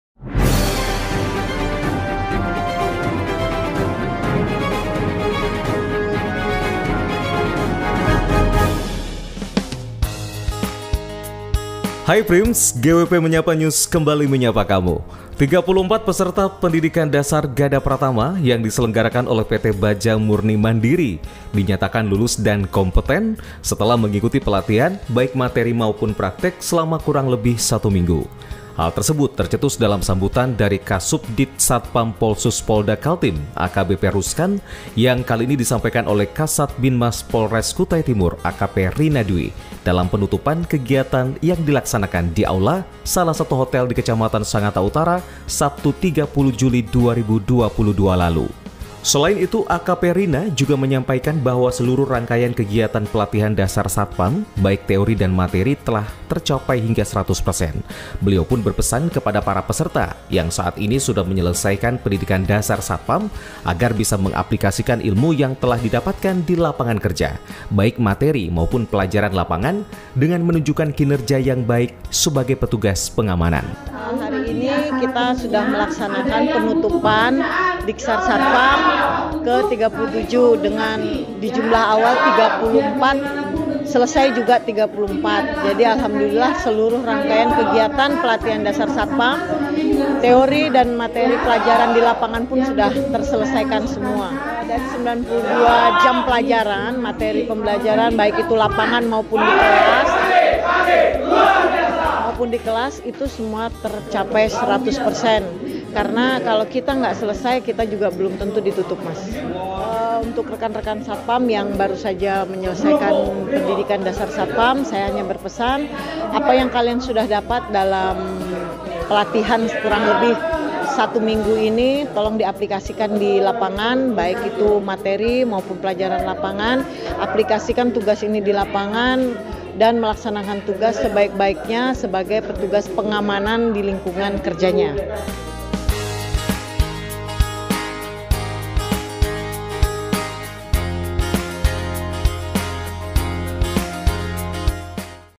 GWP Menyapa News – Peserta Diksar Gada Pratama PT Baja Murni Mandiri Dinyatakan Lulus & Kompeten
Hai Prims, GWP Menyapa News kembali menyapa kamu
34 peserta pendidikan dasar Gada Pratama yang diselenggarakan oleh PT. Baja Murni Mandiri  dinyatakan lulus dan kompeten setelah mengikuti pelatihan baik materi ataupun praktek selama kurang lebih seminggu.
Hal tersebut tercetus dalam sambutan dari KasubditSatpamPolsus Polda Kaltim, AKBP Ruskan, yang dibacakan oleh Kasatbinmas Polres Kutim, Akp Rina Dwi, dalam penutupan kegiatan yang dilaksanakan di Aula salah satu hotel di Kecamatan Sangatta Utara, pada Sabtu 30 Juli 2022 lalu.
Selain itu, Akp Rina juga menyampaikan bahwa seluruh rangkaian kegiatan pelatihan dasar satpam, baik teori dan materi telah tercapai 100 persen, beliaupun berpesan kepada para peserta yang saat ini sudah menyelesaikan Pendidikan dasar satpam, agar bisa mengaplikasikan di lapangan kerja, baik materi maupun pelajaran lapangan dengan menunjukkan kinerja yang baik sebagai petugas pengamanan.